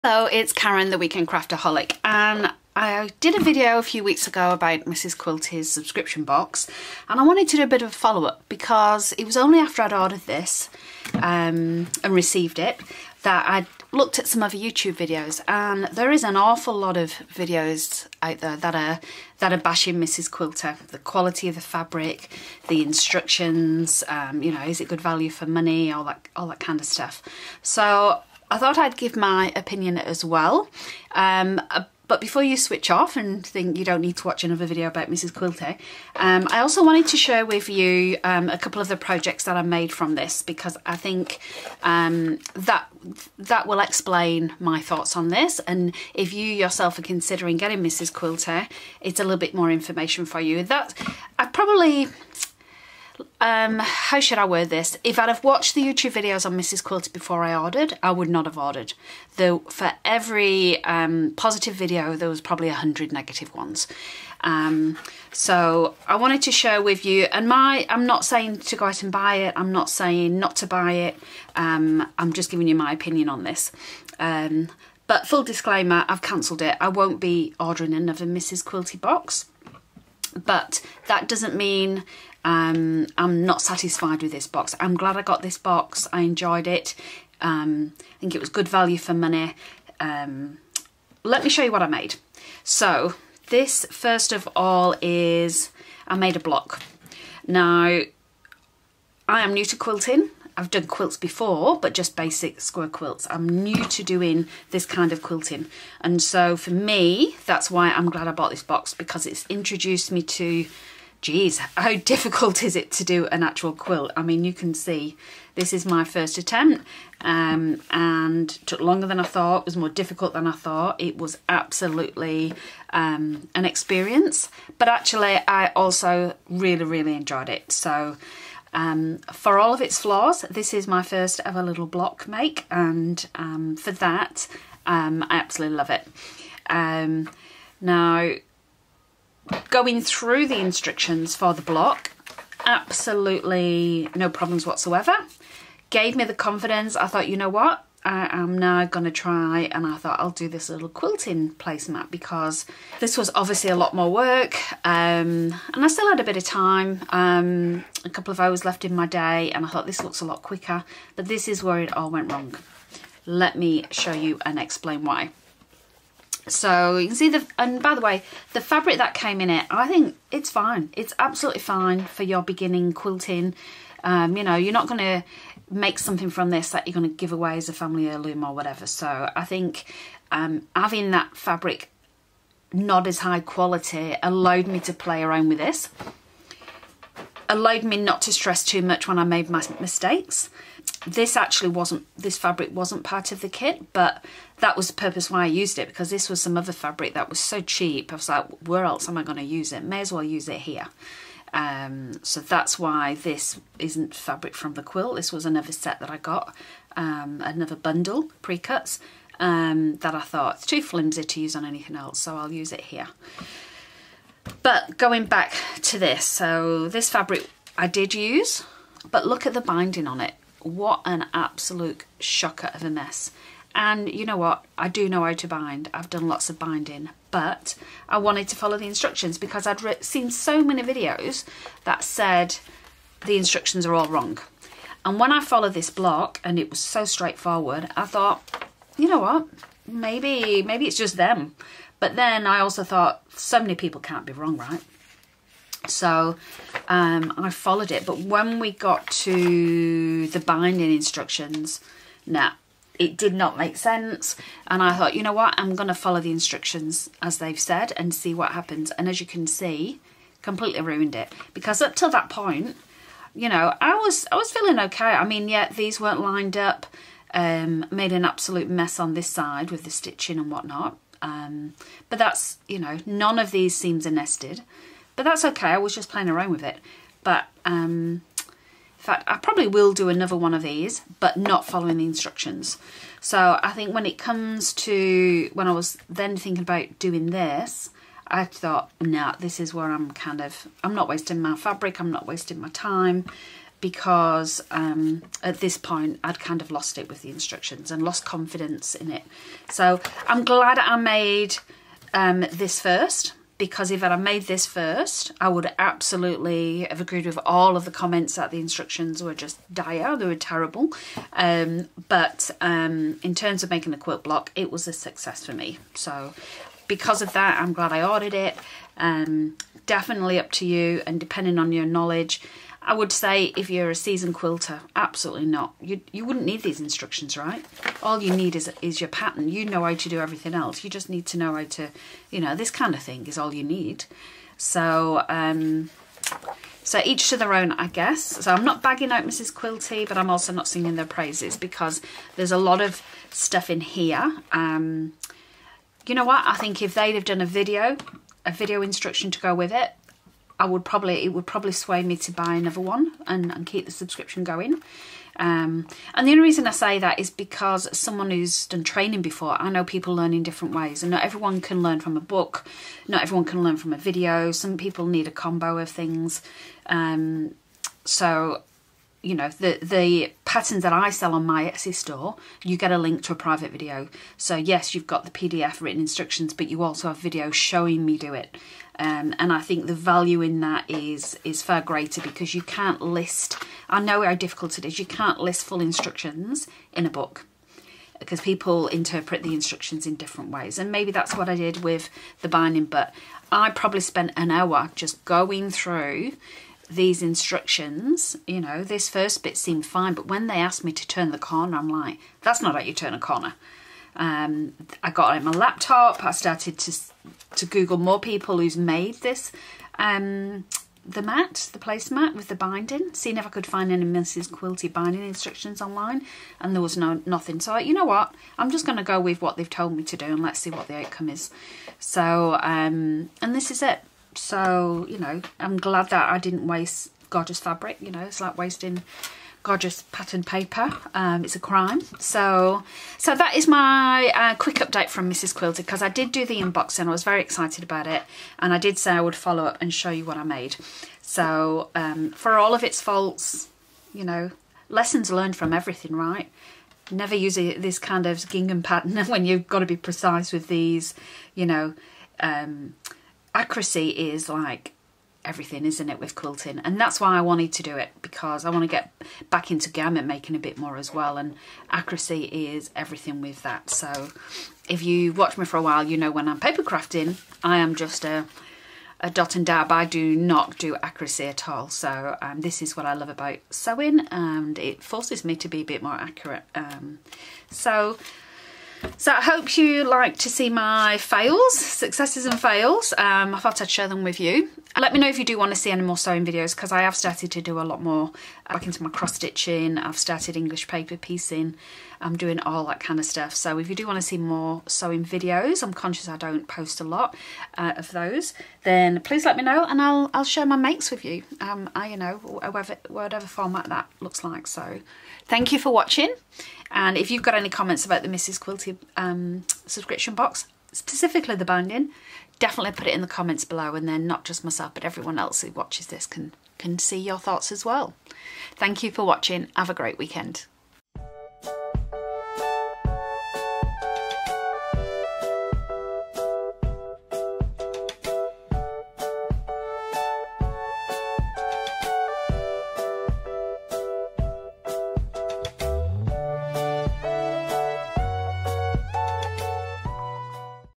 Mrs Quilty - I made 2 Projects - Here's how they turned out & my thoughts on the subscription pack
See below for further info/links

THIS VIDEO IS NOT MADE FOR, AIMED AT, OR SUITABLE FOR CHILDREN

You can find me on other social media platforms as well as YouTube: